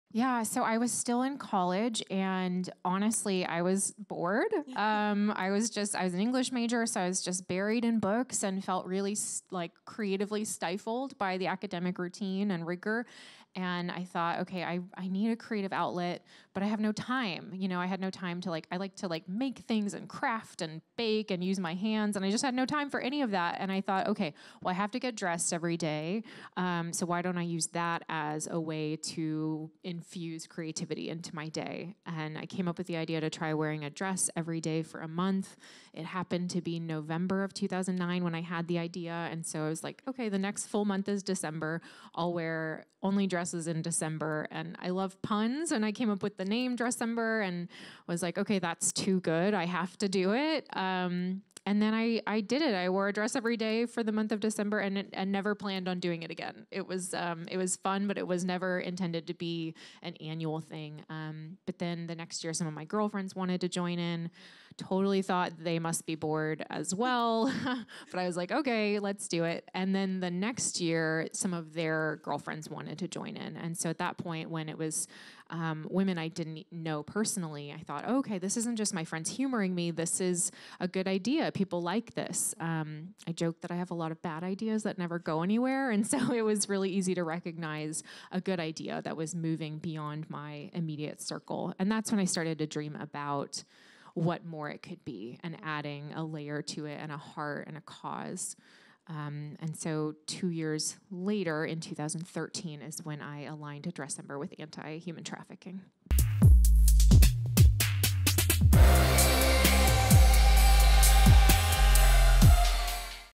Blythe Hill Shares The Origin Of Dressember
Blythe Hills discusses how Dressember came to be and how the challenge became part of inspiring awareness about human trafficking.